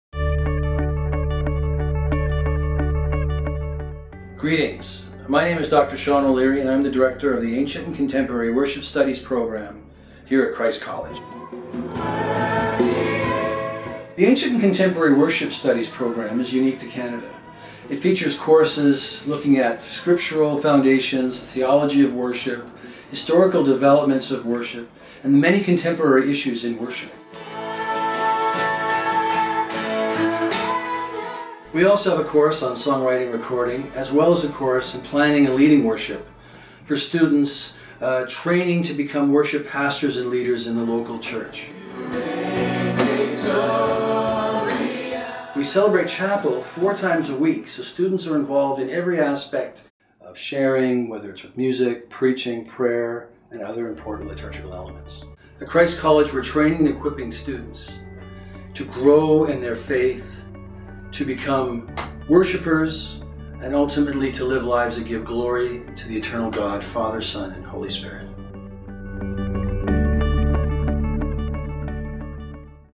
Ancient and Contemporary Worship Studies Major
Dr. Sean O'Leary explains the purpose of the Bachelor of Arts in Religion, Ancient and Contemporary Worship Studies Major at Christ College.